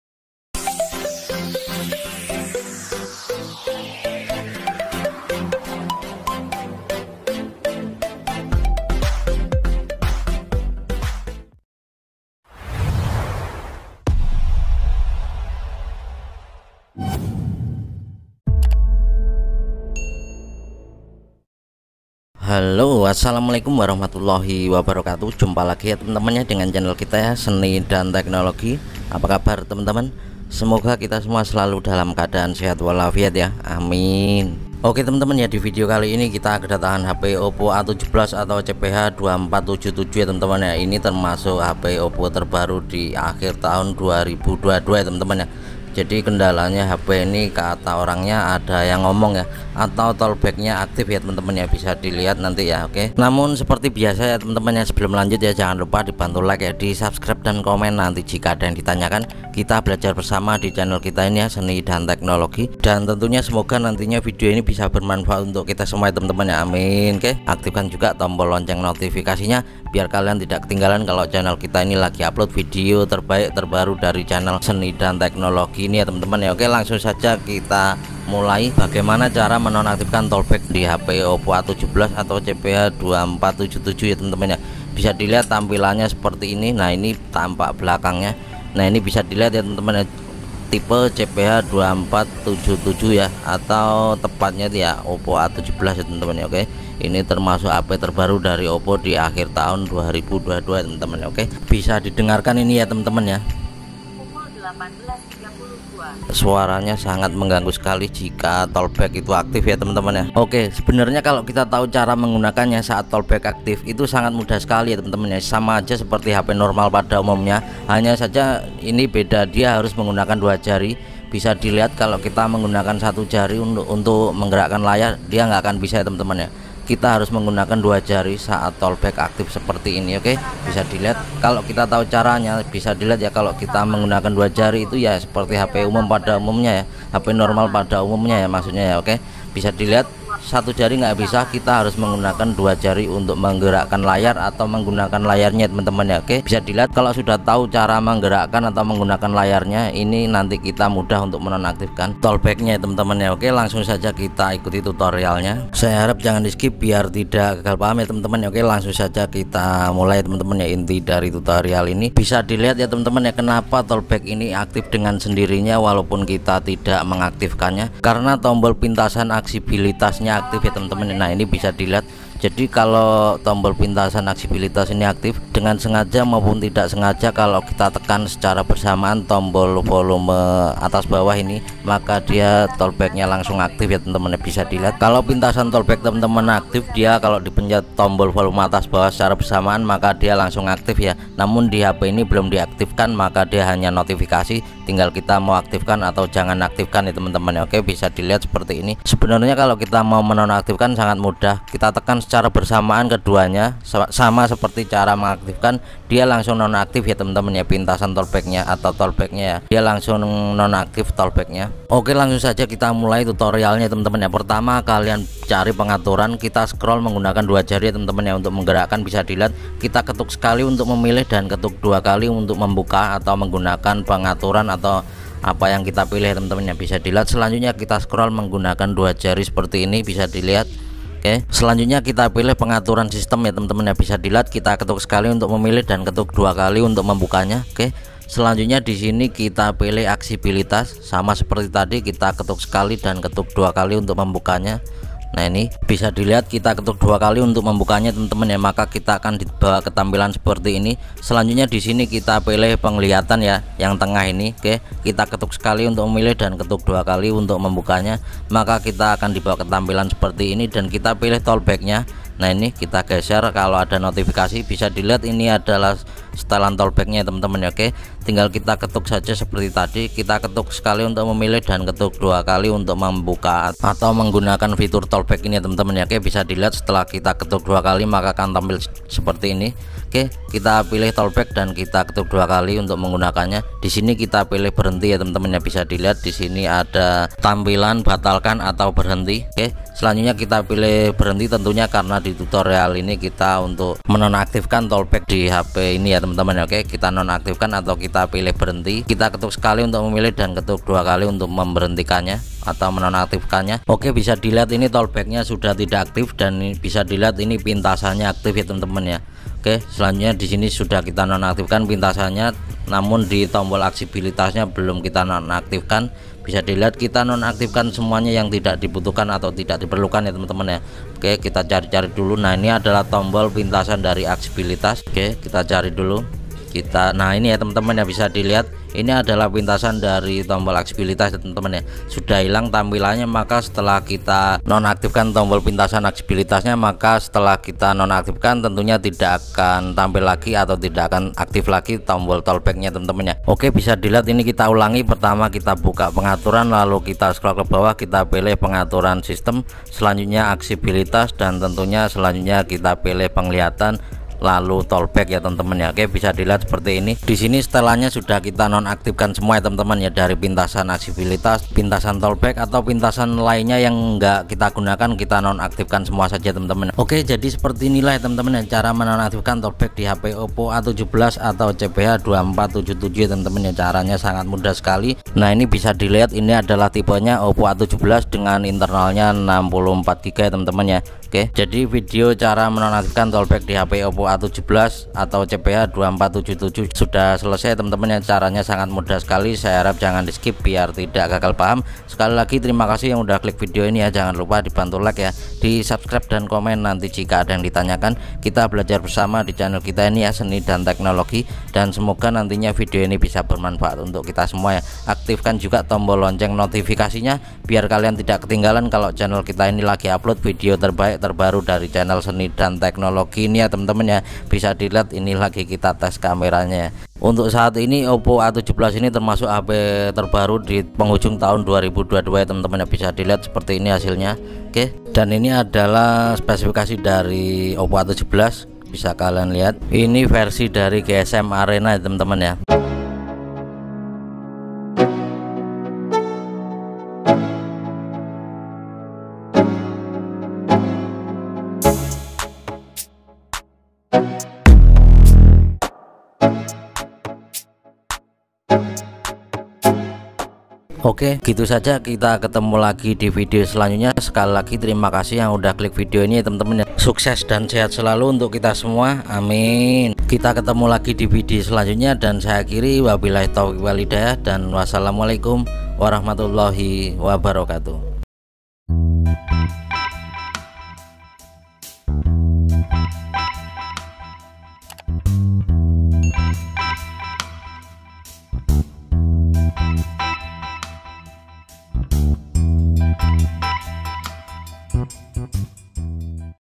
TALKBACK OPPO A17 || CARA MENONAKTIFKAN TALKBACK DI HP OPPO A17 || HOW TO DISABLE TALKBACK OPPO A17
Di video ini kita belajar bersama ya teman2.
Cara menonaktifkan talkback oppo a17 atau cara menghilangkan suara talkback di hp oppo a17, Semoga mudah di pahami dan tentunya smoga bisa bermanfaat.
🤲 amiiin...

🙏 Saweria 🥰 Boss Ku..
🙏 Ceria nya Bocil Ku..
🙏 Terima kasih yang sudah berdonasi.

@Copyright - Seni Dan Teknologi